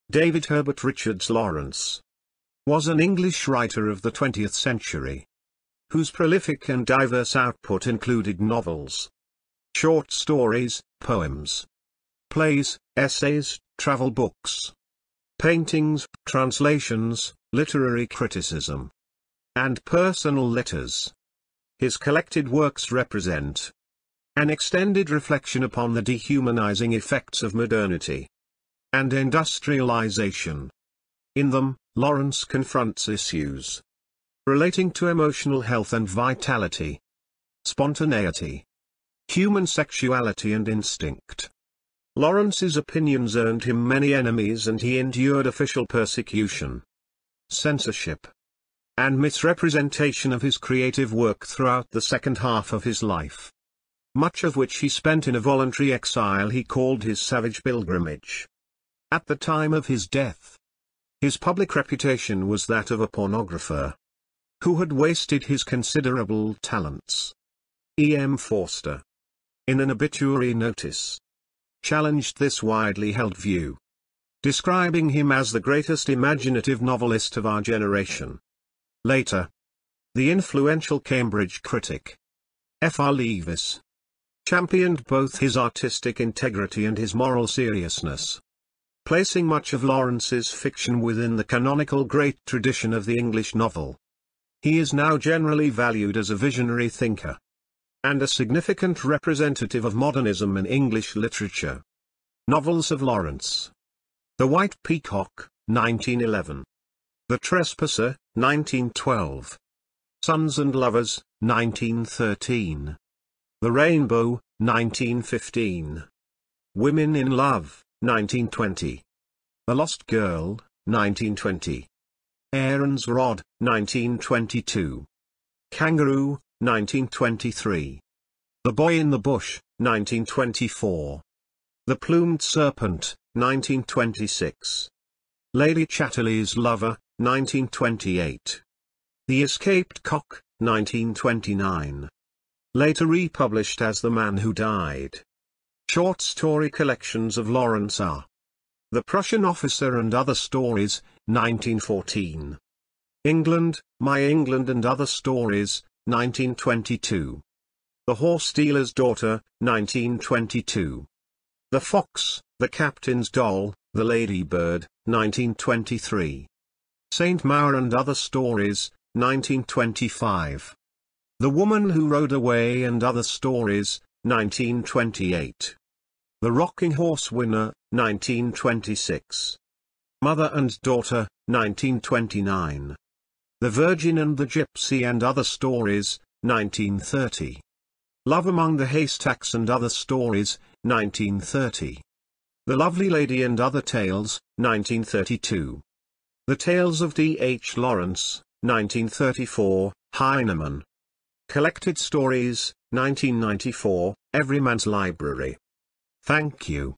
D. H. Lawrence (Biography)
David Herbert Lawrence was an English writer and poet. His collected works represent, among other things, an extended reflection upon the dehumanising effects of modernity and industrialisation. Some of the issues Lawrence explores are sexuality, emotional health, vitality, spontaneity, and instinct.